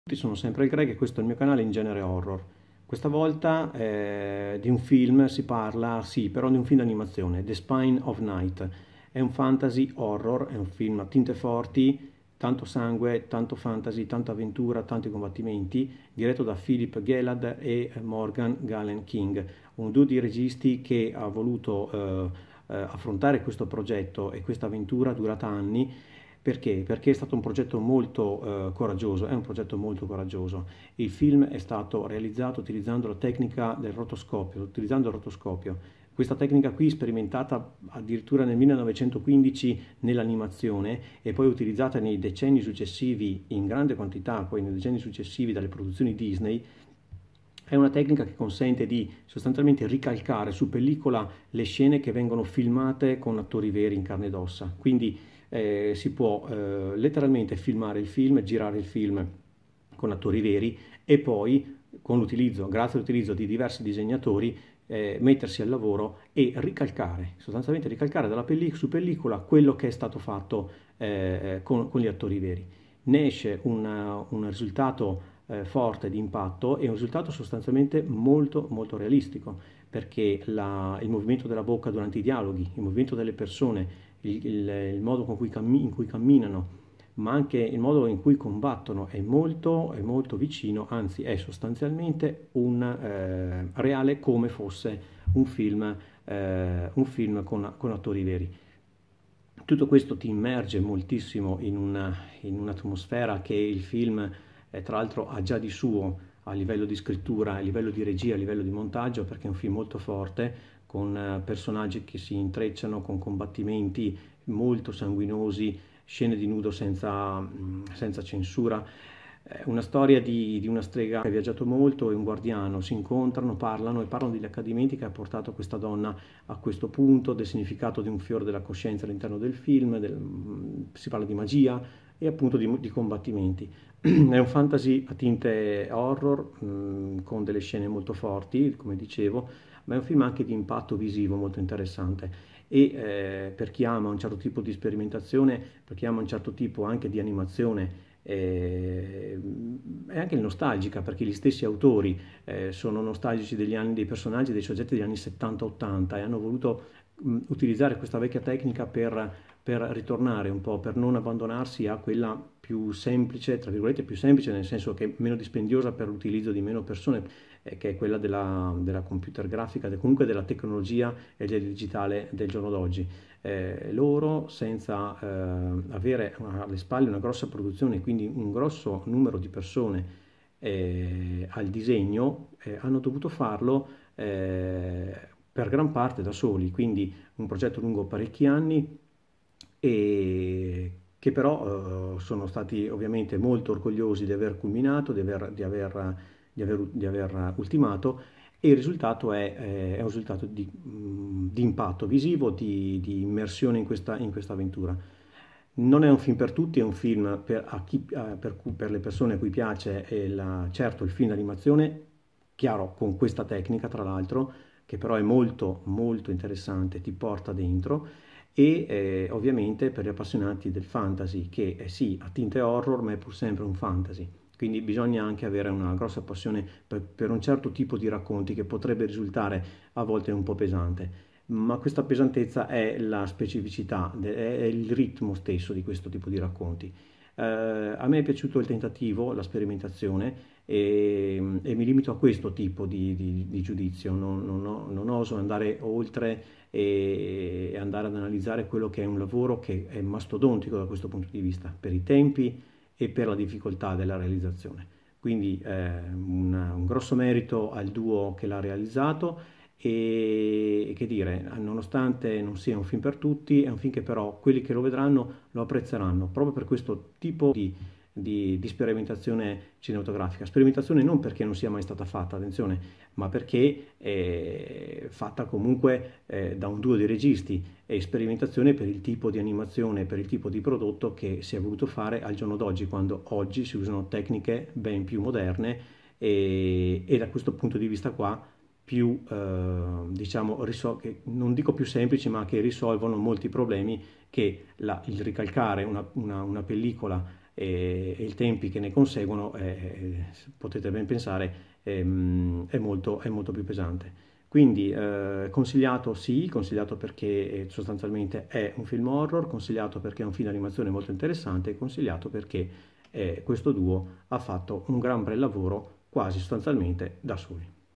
THE SPINE OF NIGHT (2021) - RECENSIONE
Fantasy horror d'animazione di Philip Gelatt e Morgan Galen King.
Disponibile sul canale Midnight Factory e in dvd e bluray sempre della Midnight Factory.

A cura di IGH InGenereHorror - in genere horror il canale di cinema horror
Recensioni film horror